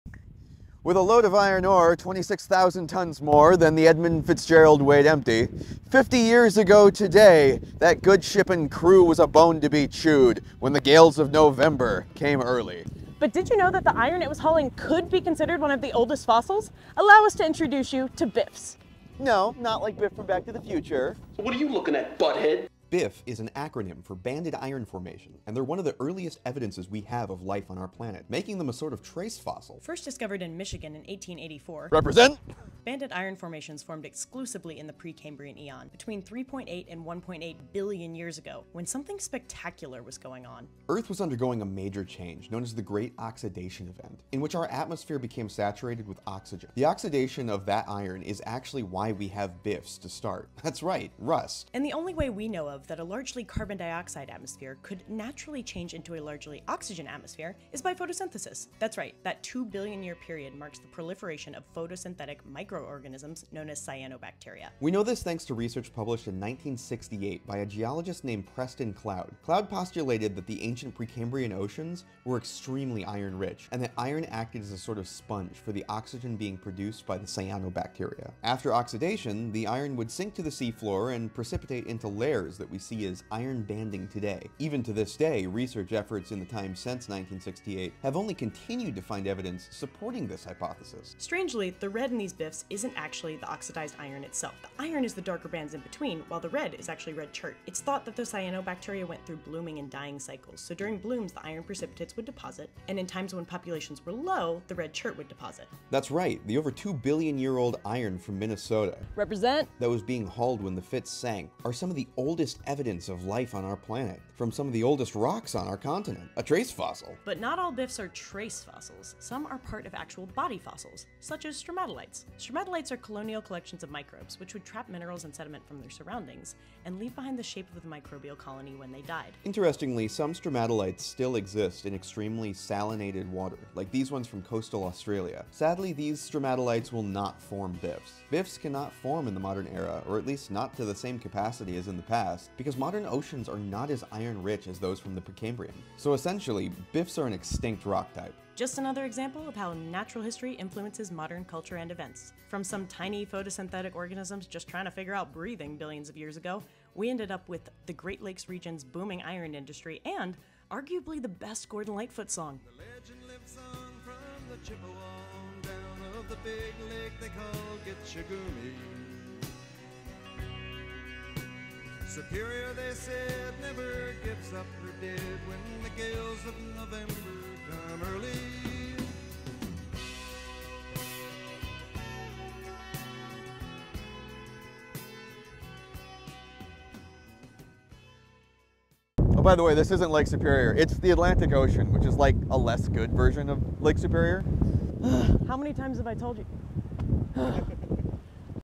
FOSSILS on the Edmund Fitzgerald??!?
Have a question? We’re happy to talk about the Fitz, rocks, and Lake Superior any time!

The Edmund Fitzgerald was an iron freighter that left a Wisconsin refinery to bring iron ore to a steel mill in Cleveland. After 17 years of service, she and her crew compliment of 29 sailors did not make it past Lake Superior due to freak storms. This story does not start on November 10, 1975 however. It starts three billion years ago!

Check us out on instagram (@geosplore, @notoriousnaturalistmax), TikTok (@geosplore and @notoriousnaturalist), Facebook (@geosplore, @notoriousnaturalistmax), and Bluesky (@geosplore)!